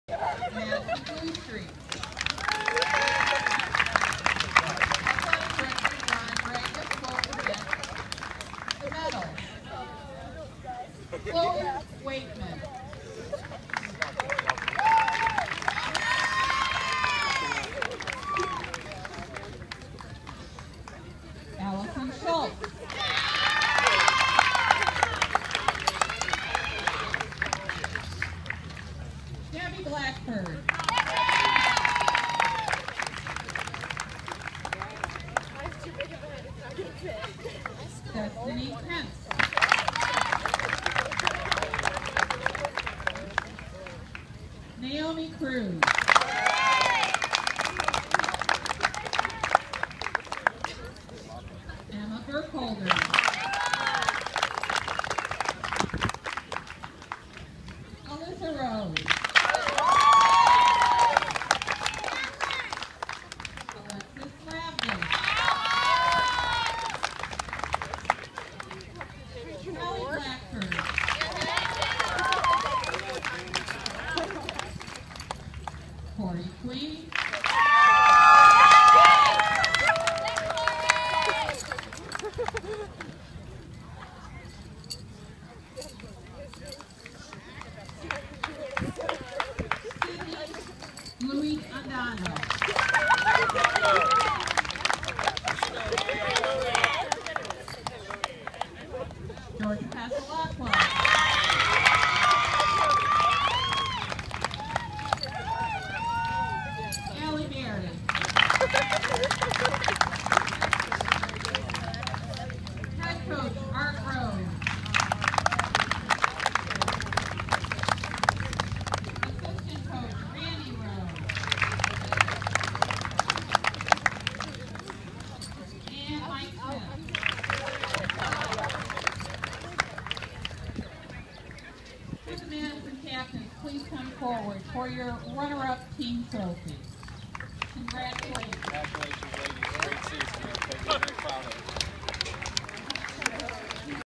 The Madison softball team accepts its district runner-up awards
The Blue Steaks fell to Willoughby South, 12-6, on May 17. The players and coaches accept their medals after the game.